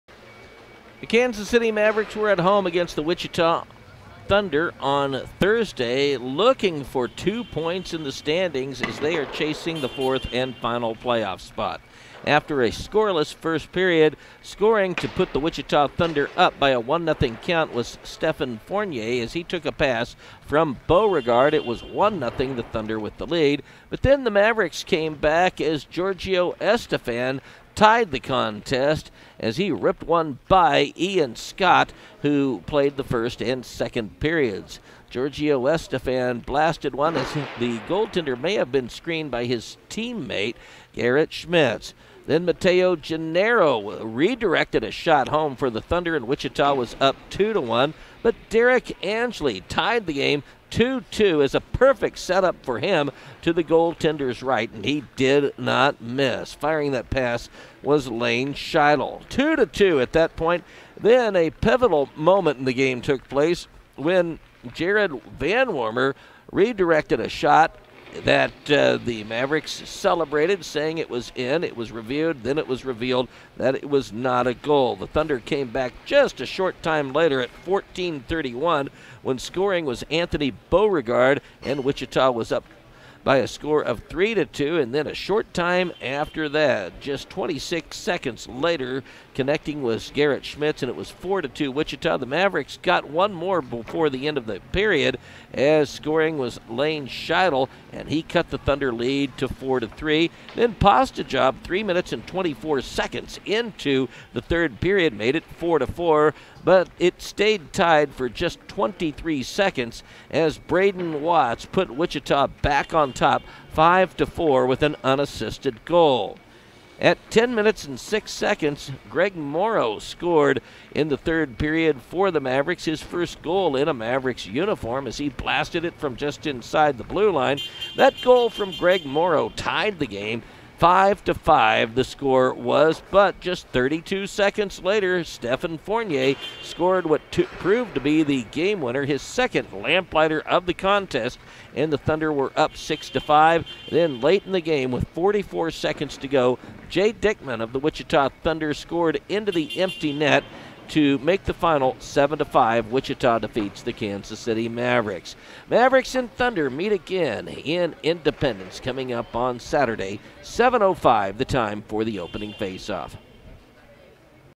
Thunder @ Mavericks 5/6/21 l ECHL Highlights
The Kansas City Mavericks hosted the Wichita Thunder at Cable Dahmer Arena on May 6, 2021.  The Mavericks fell just short by the final score of 7-5.